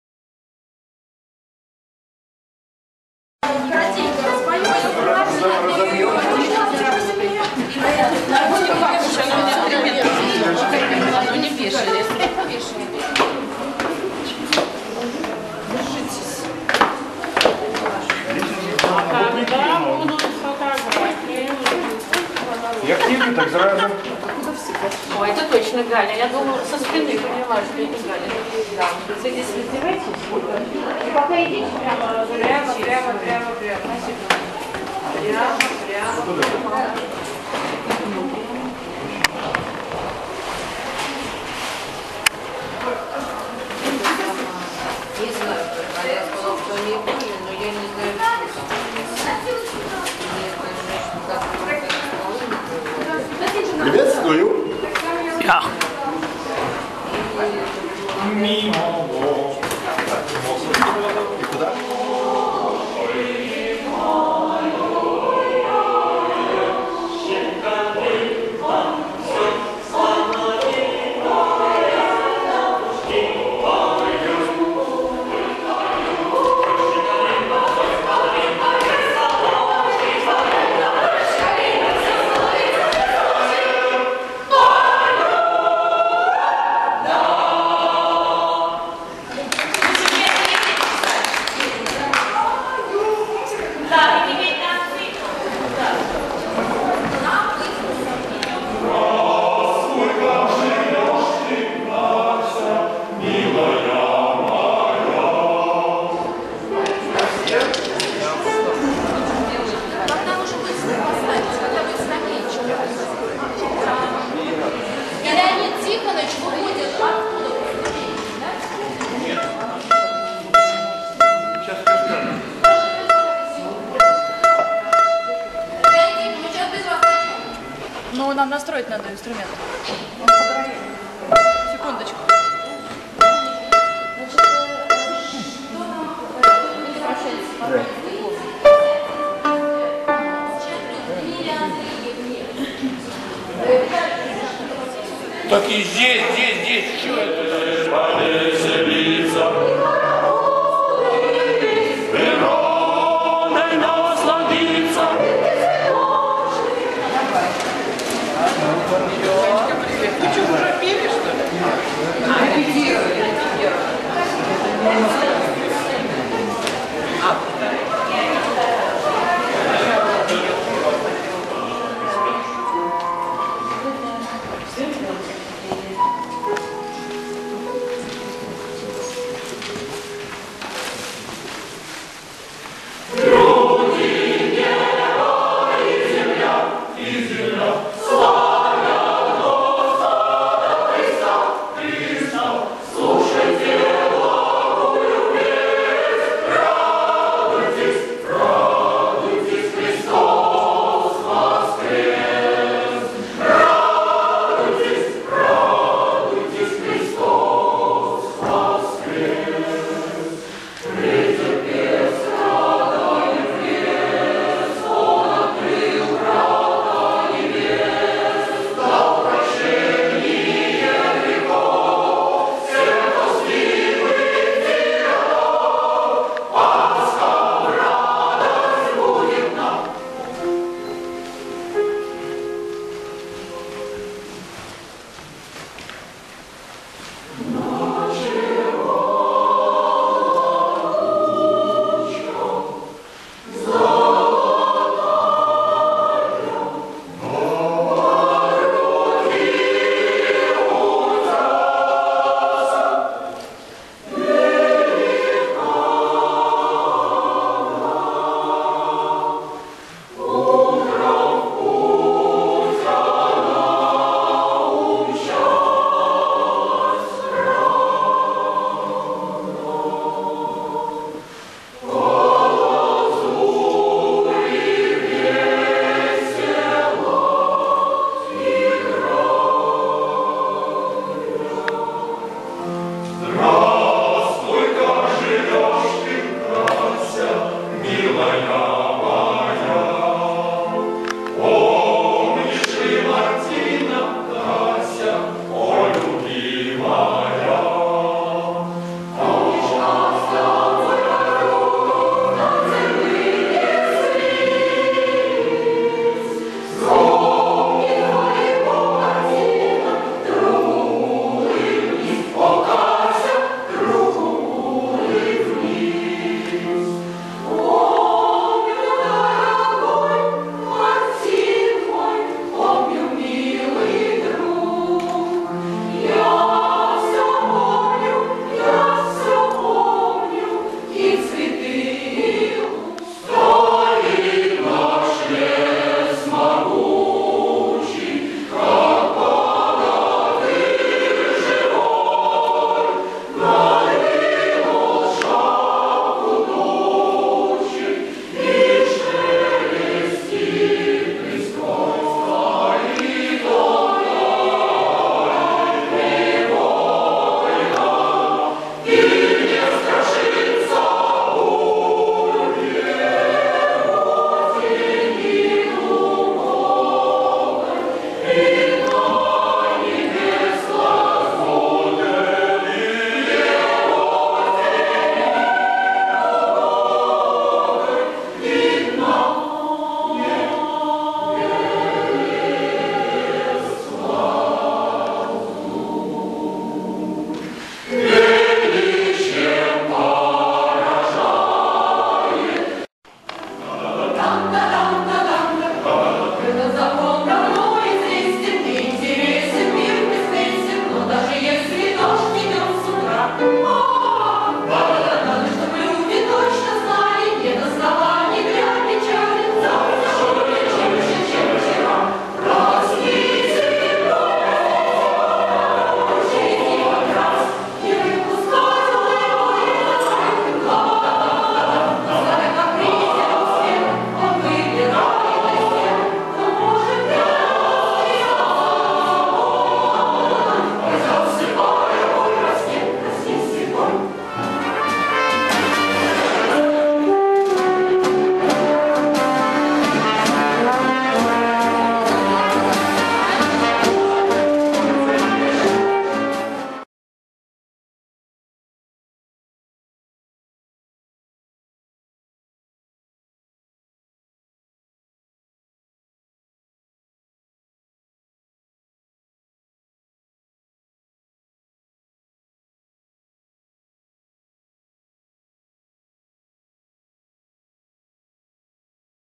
Фильм Первый Открытый Хоровой Турнир.wmv
Концерт-поединок: Камерный хор Университета точной механики и оптики; Академический хор КДЦ "Московский"